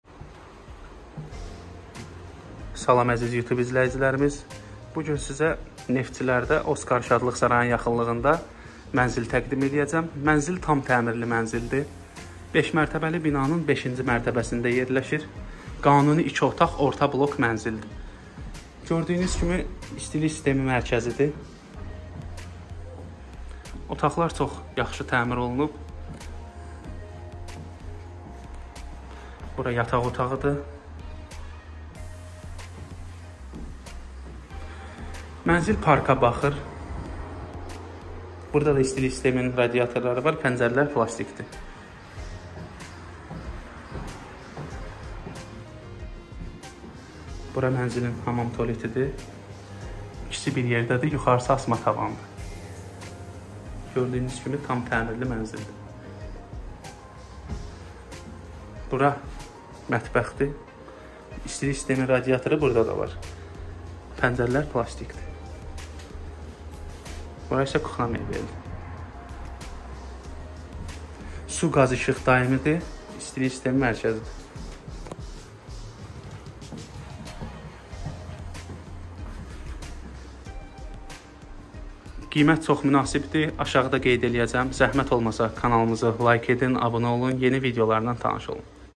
Neftçilər Oskarın yanı 2 otaq Minski Satilir 5/5 Super təmir Zahid 099-925-99-99
Nizami rayonu Nefciler metrosuna 10 deyqelik piyada mesafede yerlesen Naxcivanski kucesinde Oskar Sadliq Sarayinin yaninda yerlesen 5 mertebeli MINSKI lahiyeli binanin 5 ci mertebesinde IDEAL temir olunmus 2 Radnoy otaqli skvaznoy menzil teklif olunur yaninda mektebler baxcalar park Araz super market her bir sey yaxindir su qaz isiq daimadir sened KUPCA isdilik Merkezidir
Xidmet haqqi 1%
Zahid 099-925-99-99